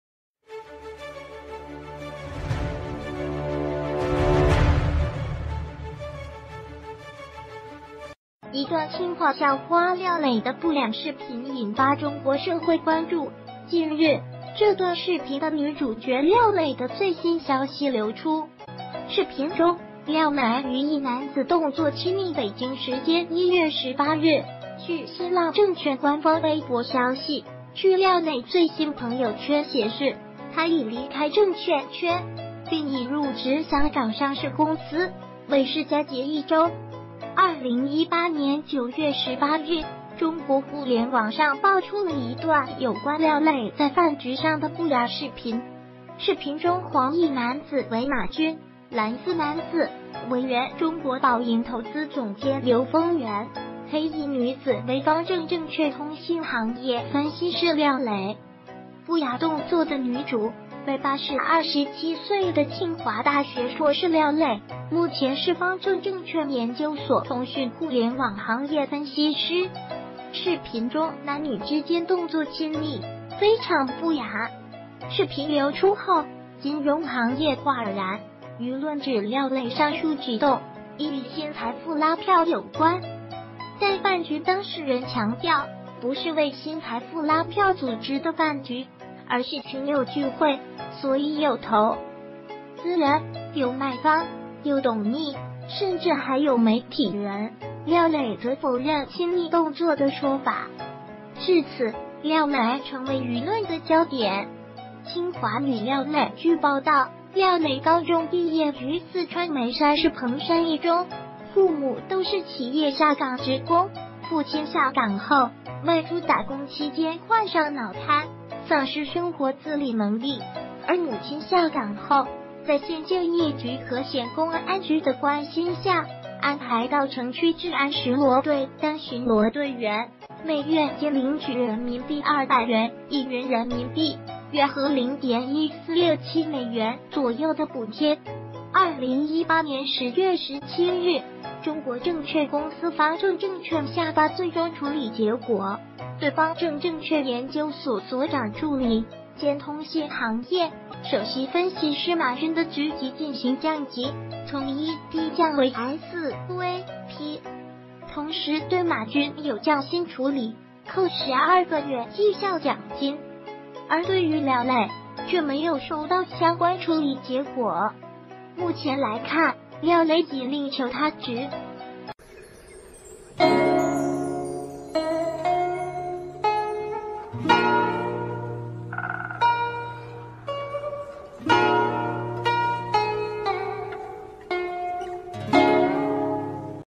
曾陷不雅視頻的清華校花重歸公眾視野
一段清華校花廖蕾的不...